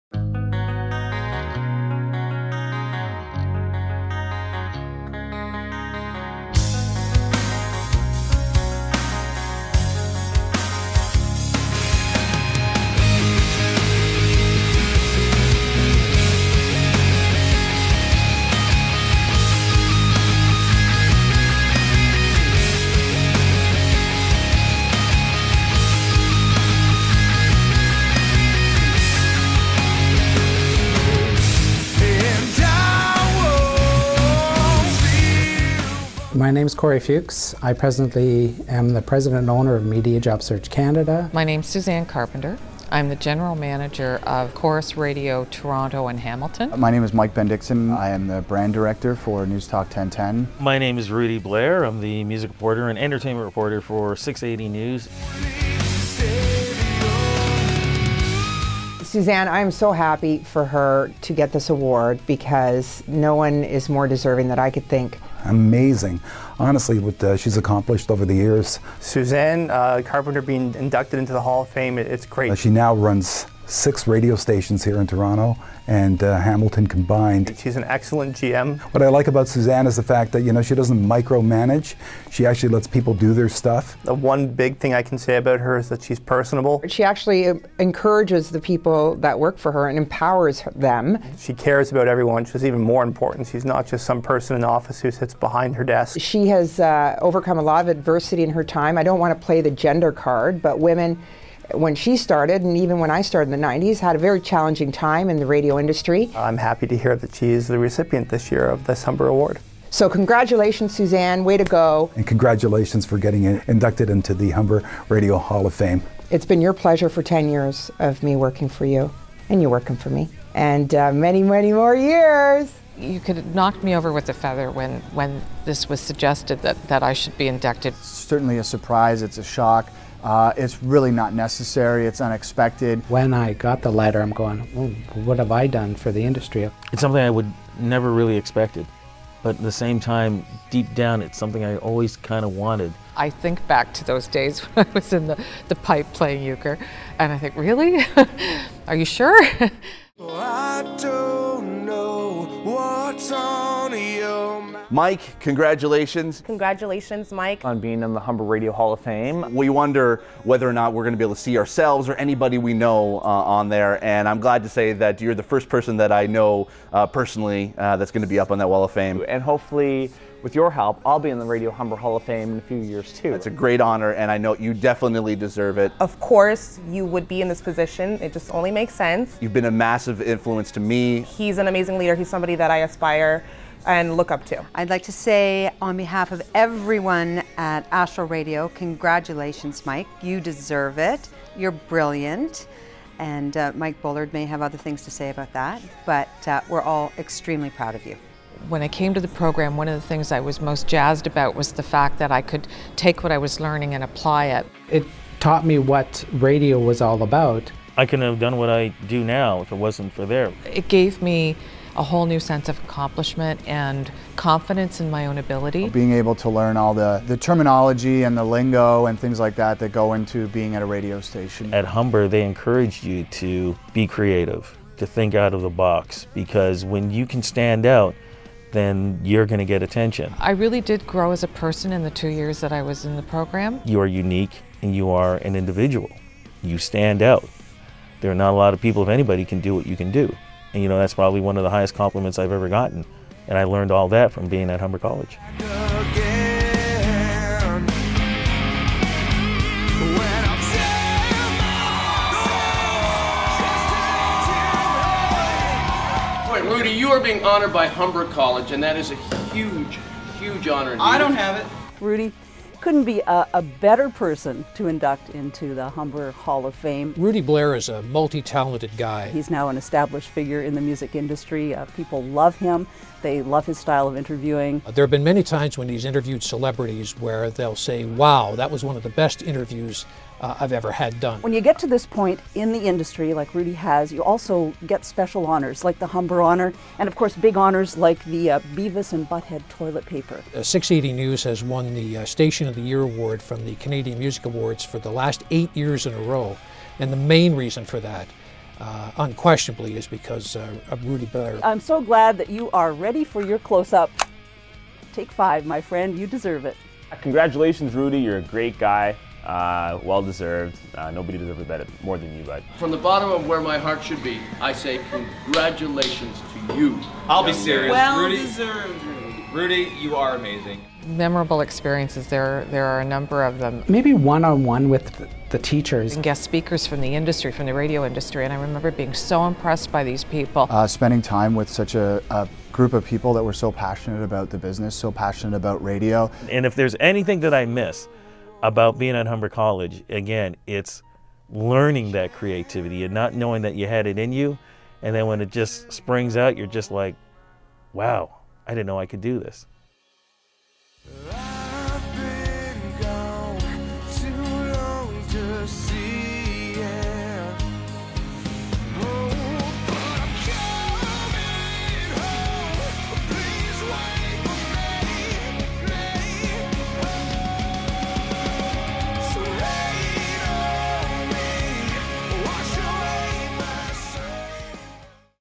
Humber Radio 40 Hall of Fame Inductee Movie
May 17, 2012 - Humber Radio Celebrates You! Profiling the 2012 Radio Hall of Fame inductees: Suzanne Carpenter, Mike Bendixen, Rudy Blair, and Corey Fuchs. Thanks to Humber BRTV crew Stefanie Sannuto, Marcel Baker, Michael Johnson and Karen Young!